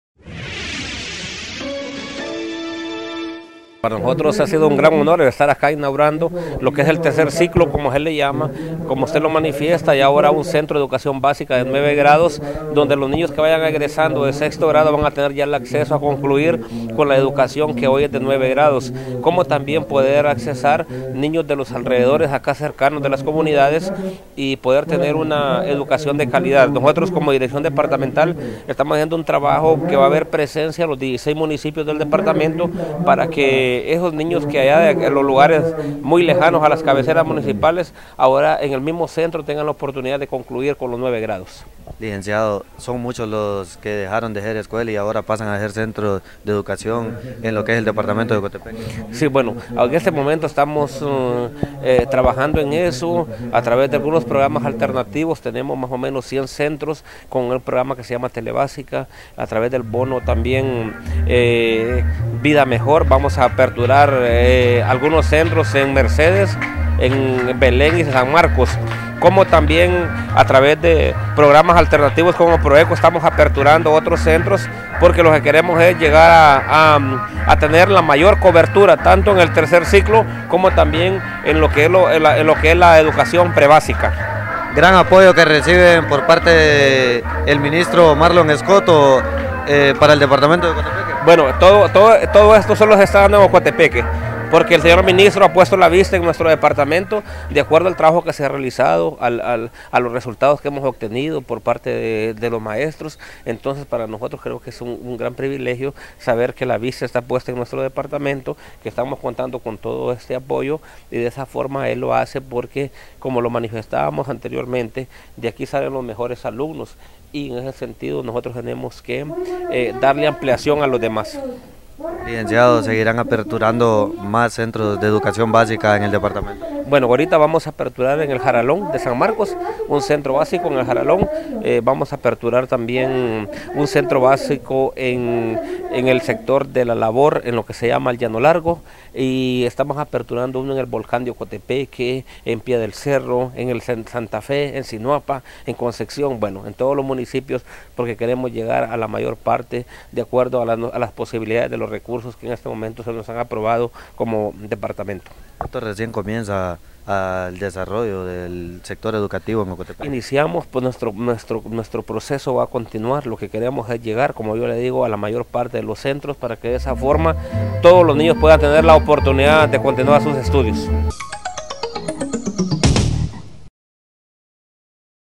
Avance Noticioso San Marcos Tv_13 Febrero 2015_Edición 4
Lic. Mauro Hernández, director departamental de educación informando acerca de la gran inauguración el día de hoy del Centro de Educación Básica en el municipio de San Francisco del Valle…